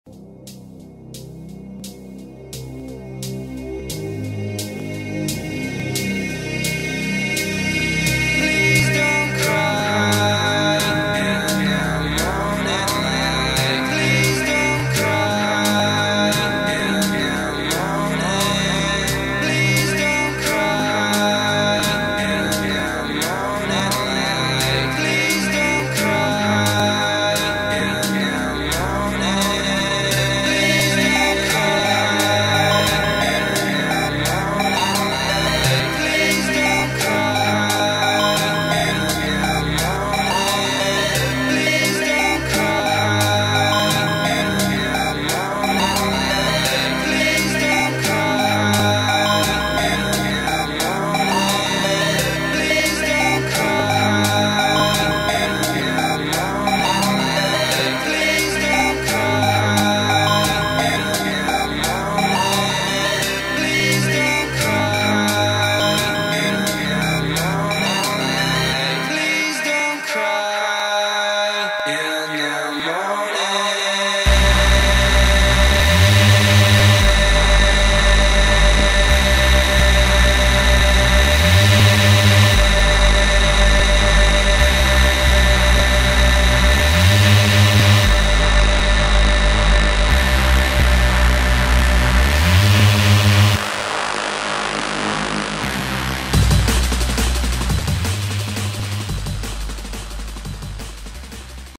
RIP - Sara and Eso (American Bulldog and Boxer buddies)
2 of the best buddies in the world passed away far before their time and far too young. RIP sara and eso, you will both be missed.